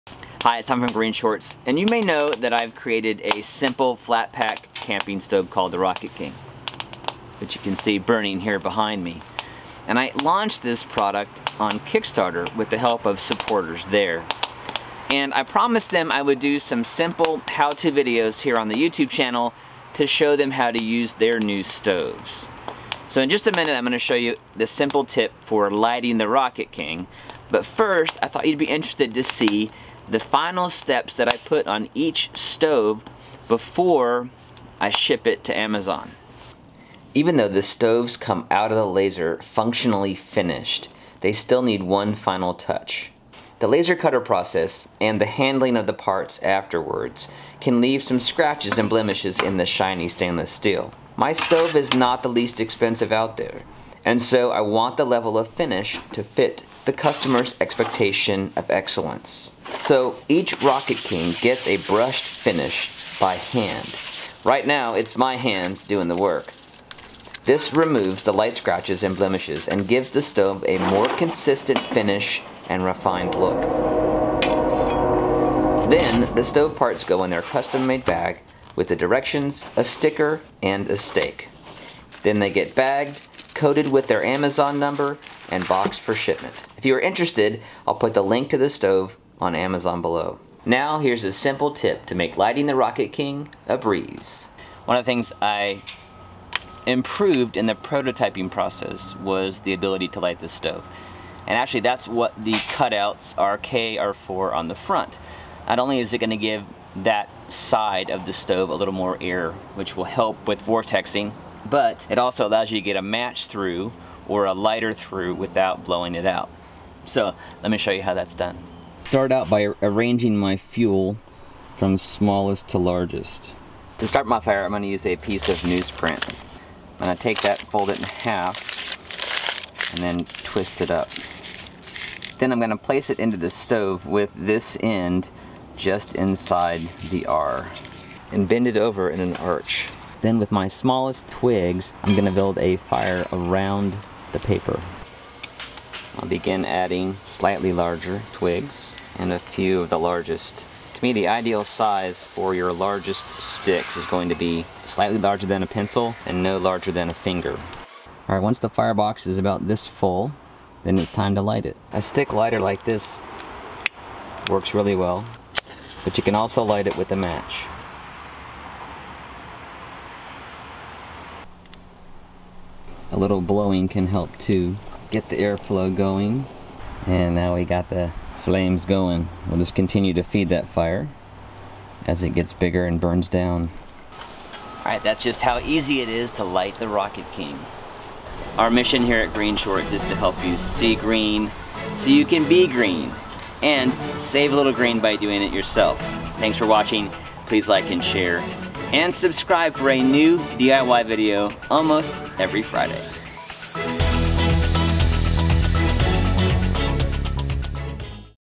How To Light The Rocket King Camping Stove
As a promise to my Kickstarter supporters, here's a short video on how to light the Rocket King Camping Stove. I also show the final brushed-finish detail I personally add to the parts of each stove before I ship a batch to Amazon.

The Rocket King Camping Stove is my design and is laser cut from 16 gauge stainless steel. It is 9 inches tall and 6.25 wide in both dimensions. It weighs just under 3 pounds with the included bag and anchor stake.

A rocket incorporates a rising air flow through the fire to boost the burn, for a hotter and cleaner consumption of the fuel. Fuel is small sticks and twigs...a sustainable found-fuel which can be found at your camp site and doesn't need to be packed in or carried out.

I also have plans available for the stove, if you'd prefer to DIY.

Main GreenShortz Channel: Short Videos About Easy Ways to Be Green